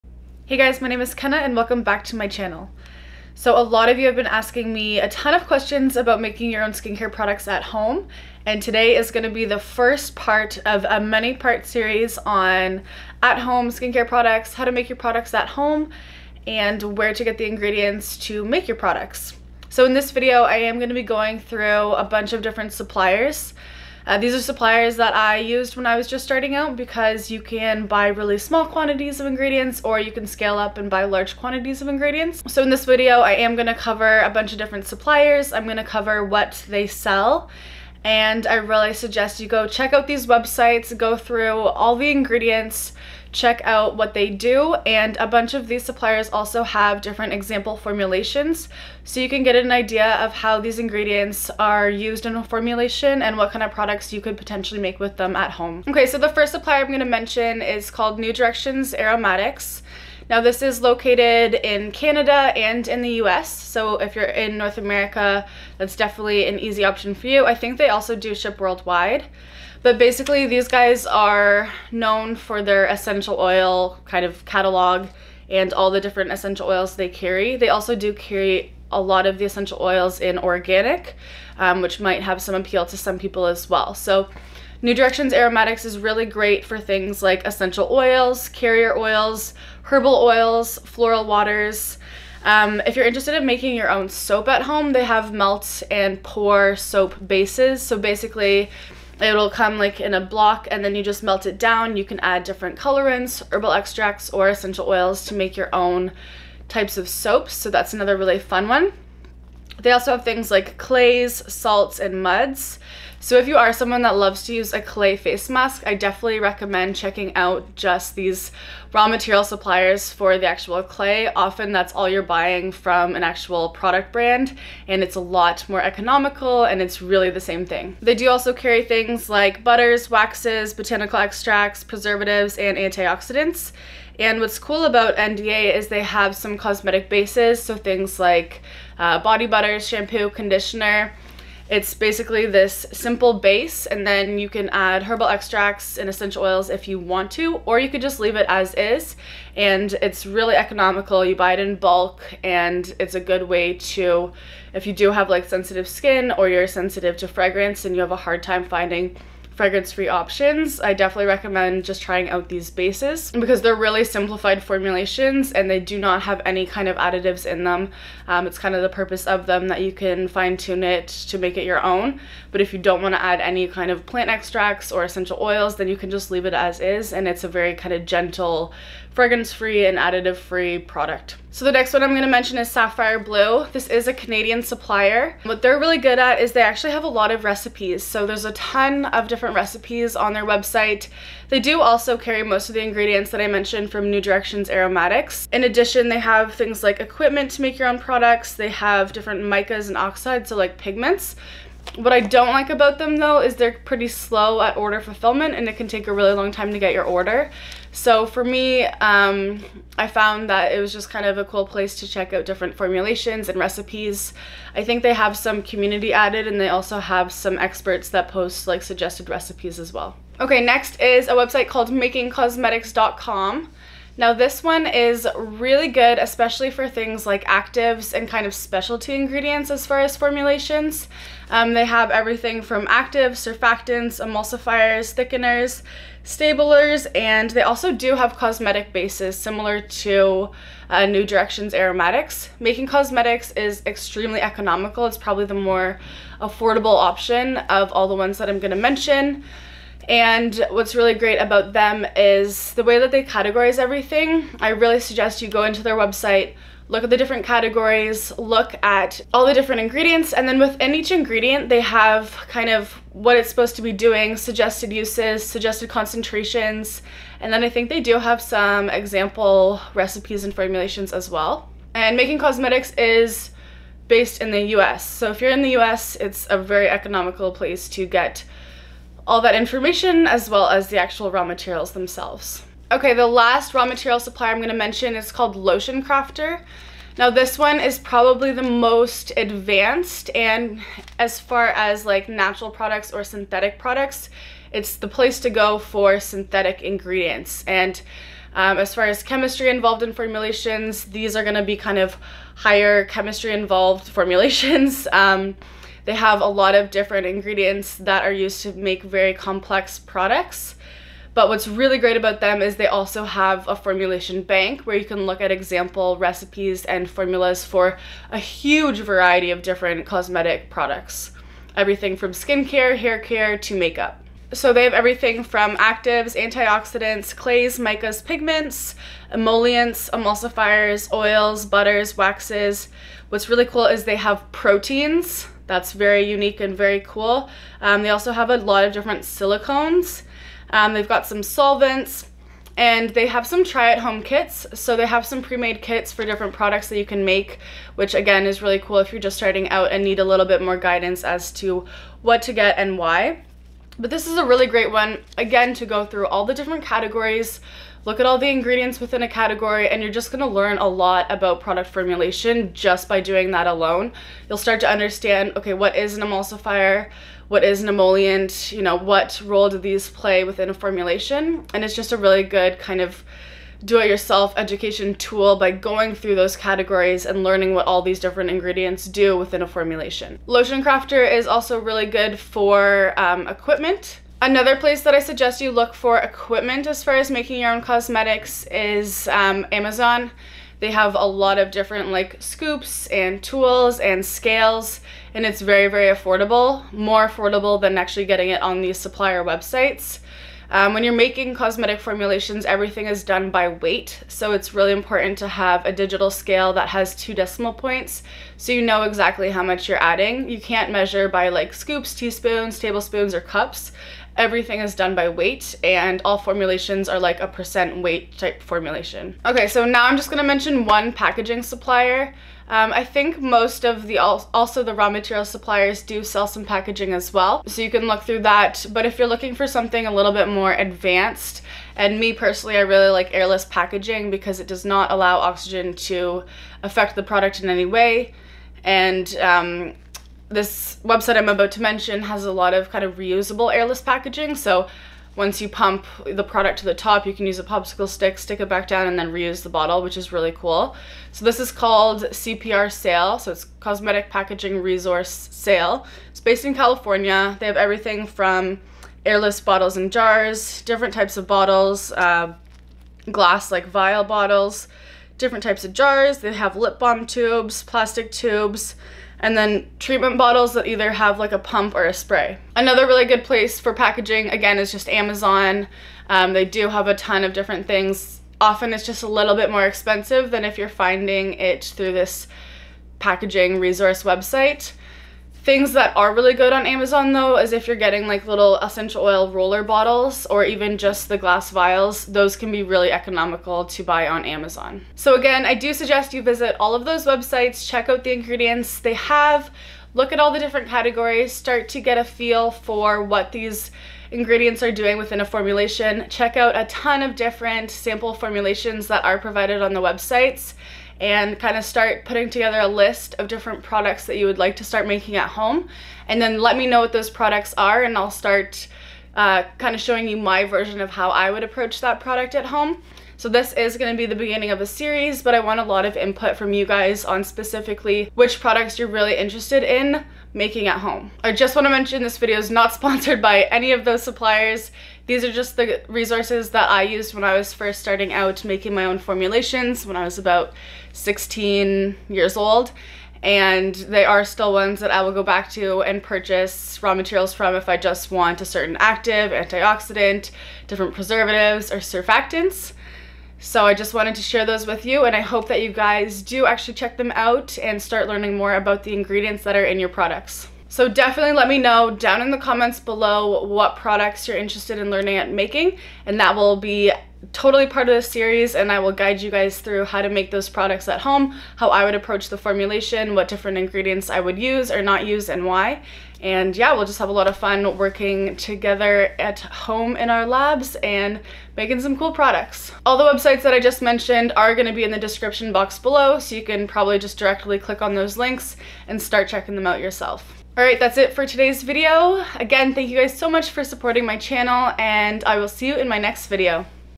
MAKE YOUR OWN SKINCARE, HAIRCARE & MAKEUP | Ingredients, Equipment and Packaging Suppliers
Hi sciencebabes ! Today's video is part one of a infinitely long series of how to make your own skincare and cosmetic products at home. Comment what products you are most interested in learning how to make!

Packaging:
Amazon